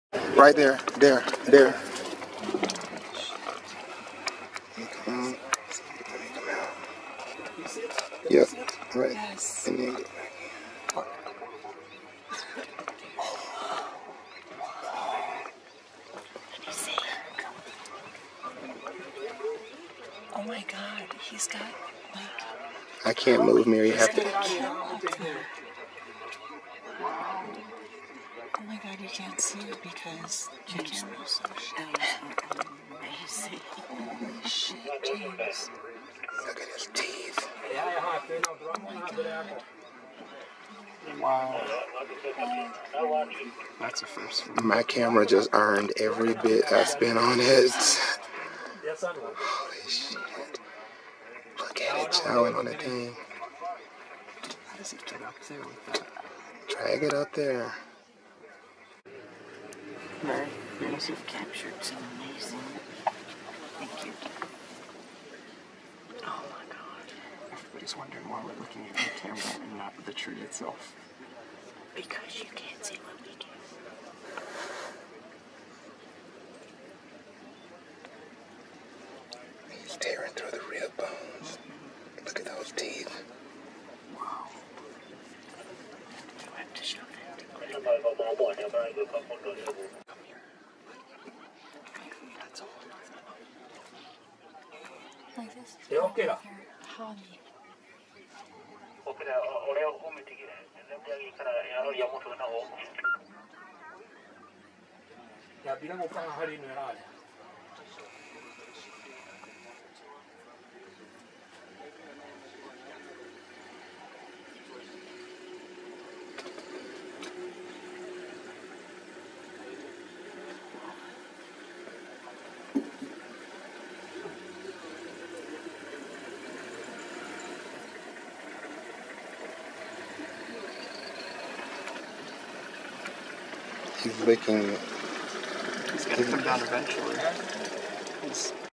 Leopard Eating Antelope In Masai Mara Tree
September on a Masai Mara Game Drive this leopard was found with an Antelope in the tree.  There was a 2nd Leopard on the group sleep with a hyena stalking the base of the tree for scraps. Shot with a Nikon Coolpix P950